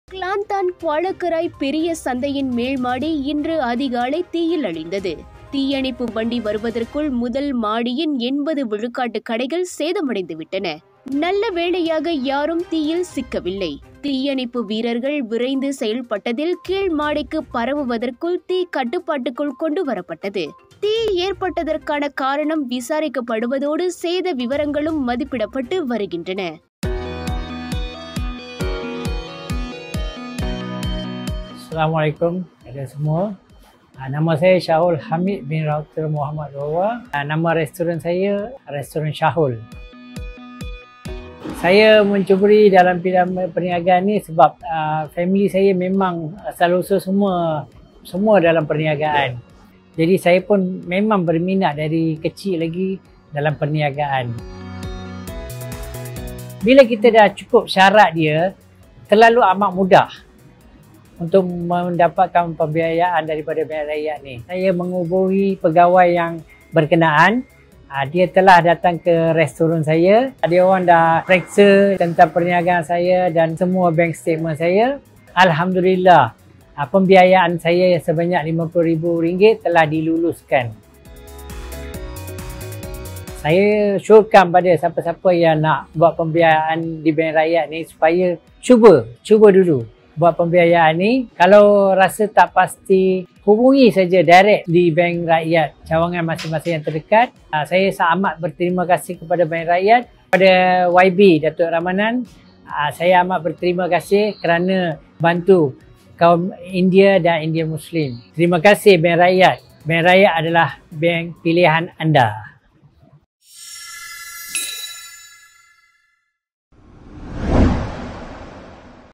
14/10/2024 : Tragedy! Fire breaks out at Kuala Krai market
Tragedy! Fire breaks out at Kuala Krai market